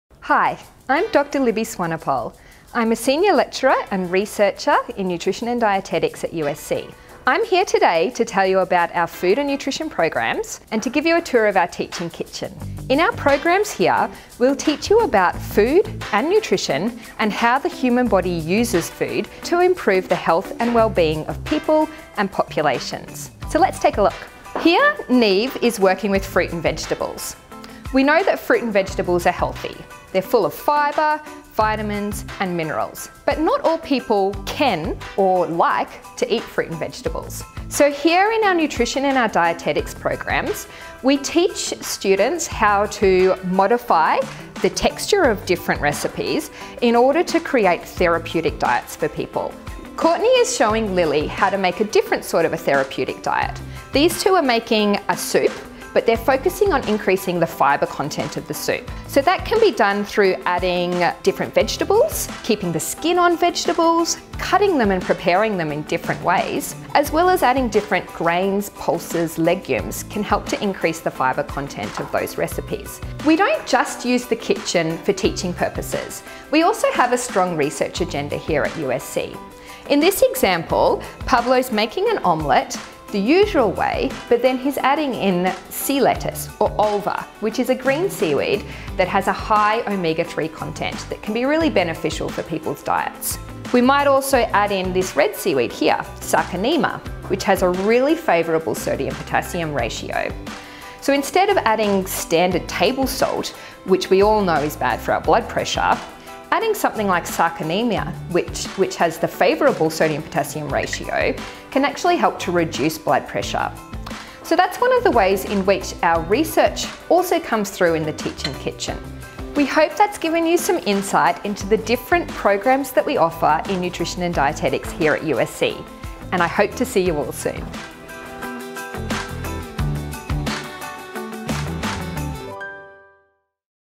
USC Nutrition and Dietetics facilities tour
Take a tour of USC's Nutrition and Dietetics facilities!

Senior lecturer, Dr Libby Swanepoel, shows you through the kitchen facilities  and some of the information you'll be learning through your course.